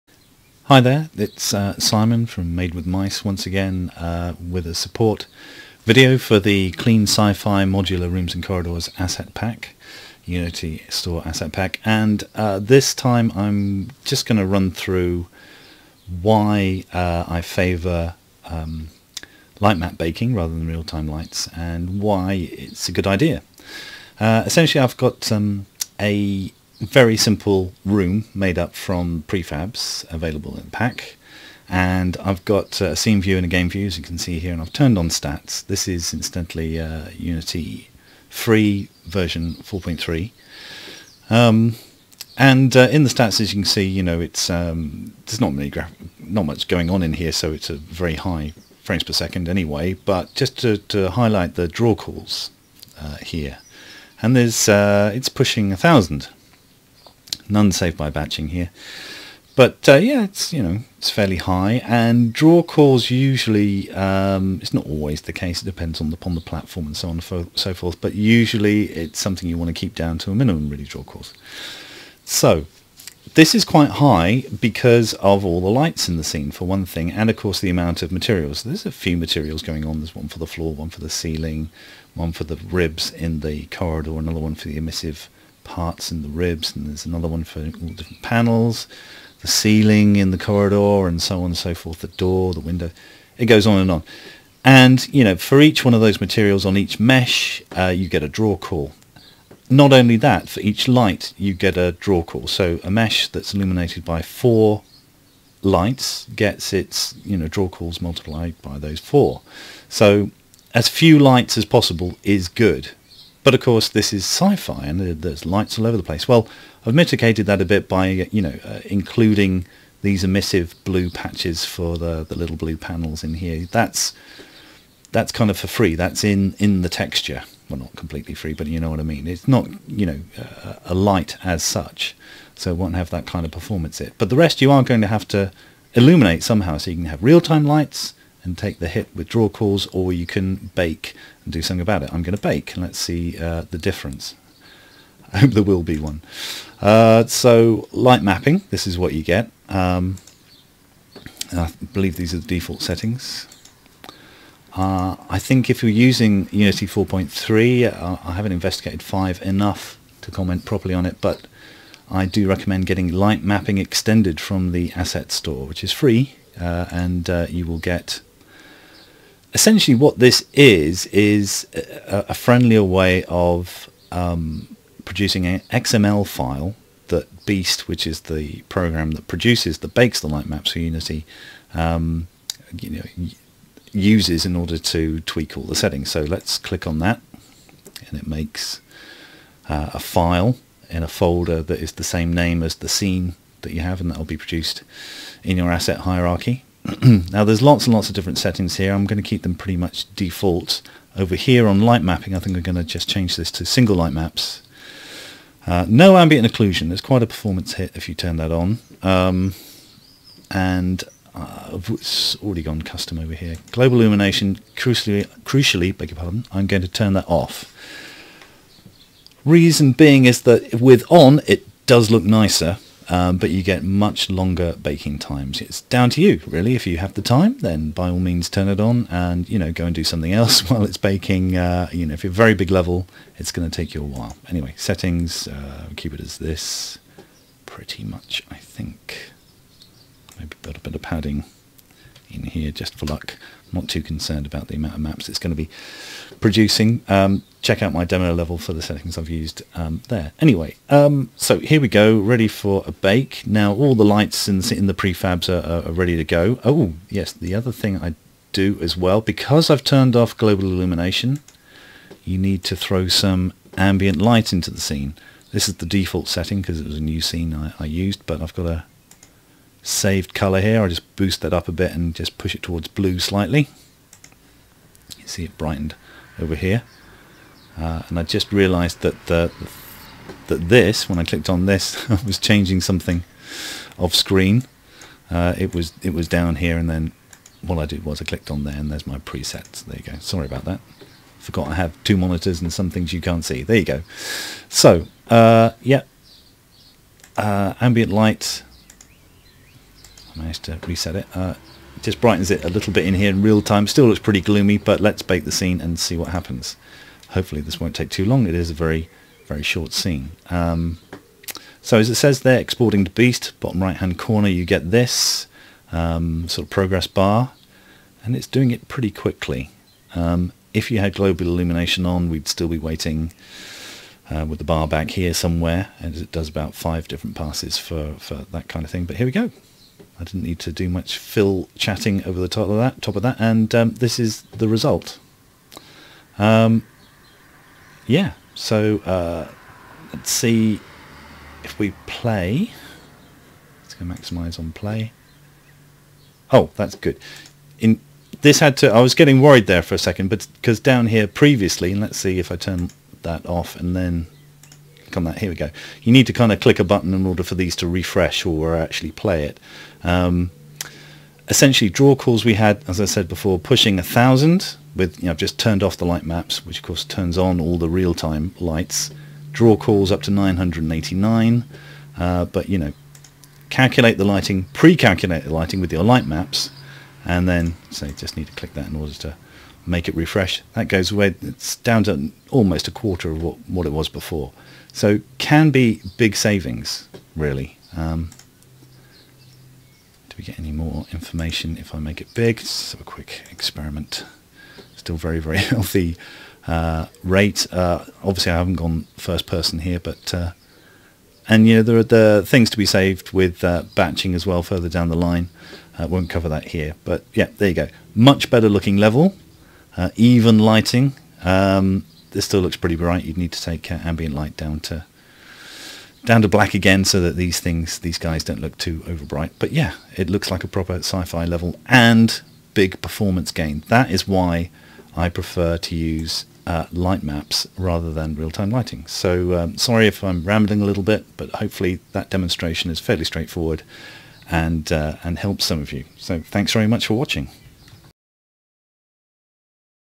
Lightmapping and draw calls
A support video for the Clean Sci Fi: Modular Corridors and Rooms asset pack available from the Unity Asset Store here -